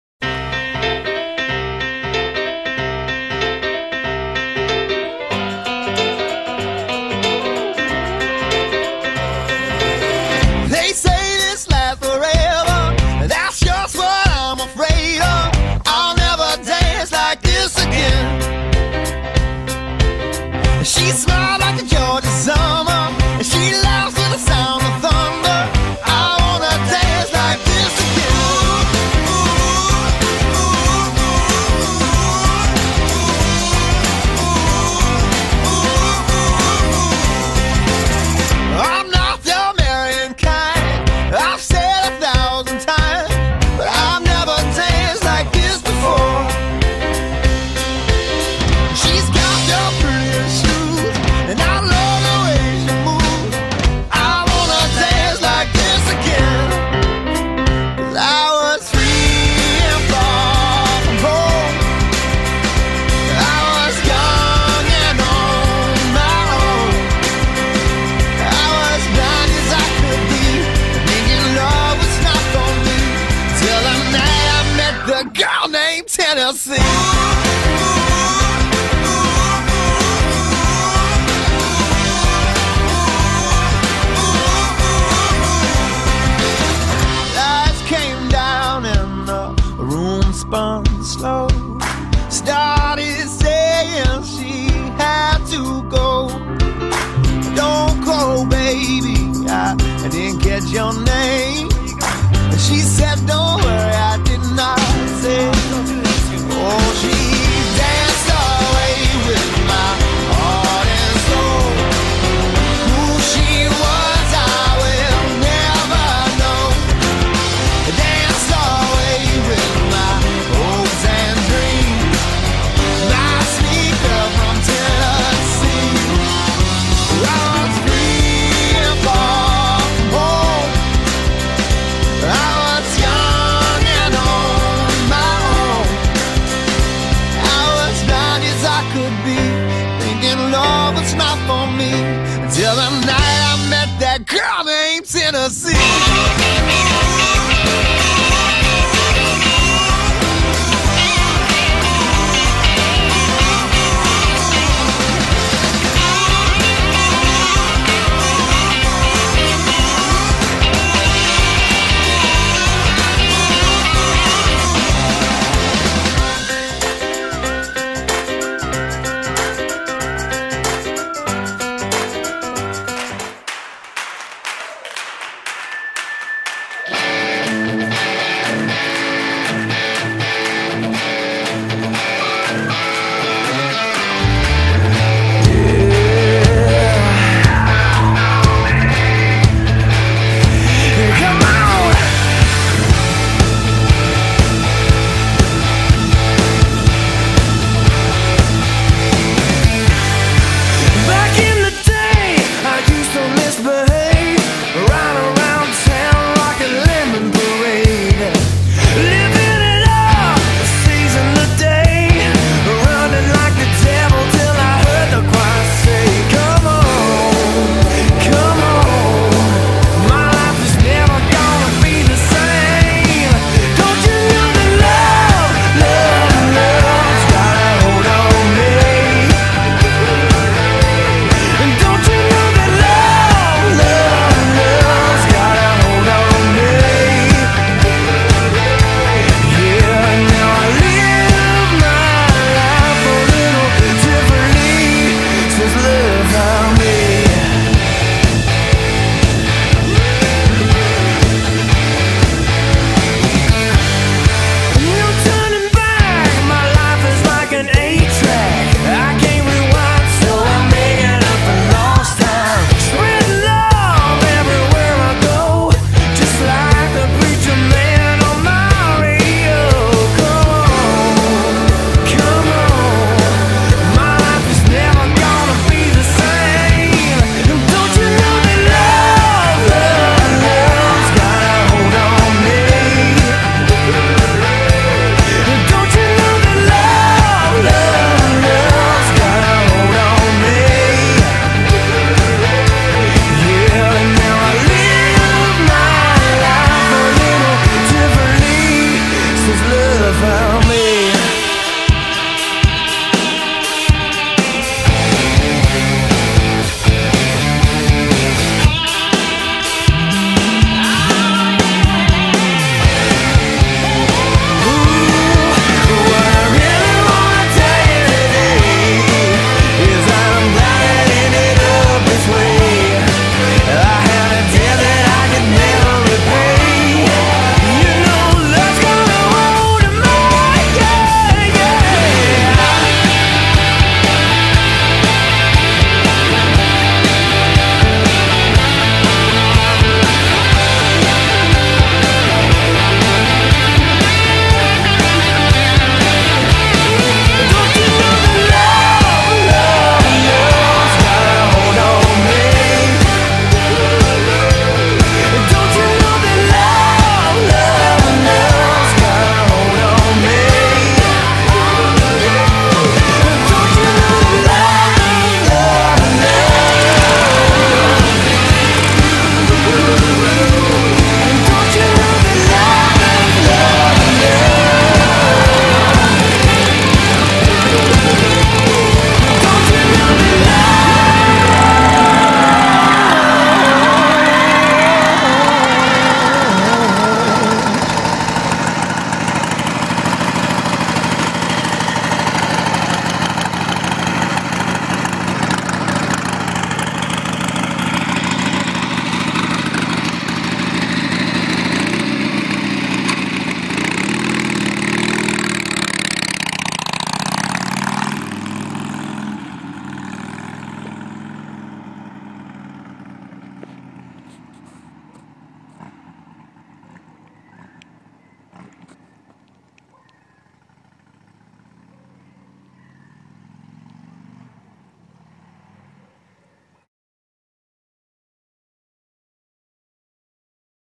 76Iron.m4v
6 month basket case build beginning Nov 2011 and ending on April 2012.  Started with a truckload of parts and stripped down to bare metal and finished a 1976 Harley Davidson "Ironhead" Sportster 1000cc.  Copper flake paint with Ironhead graphics, 15" Apes, Bobbed rear fender, Larosa Seat, Porkster style tank, Pingel High flow valve, S&S Super E Carb, Andrews PB+ Cams,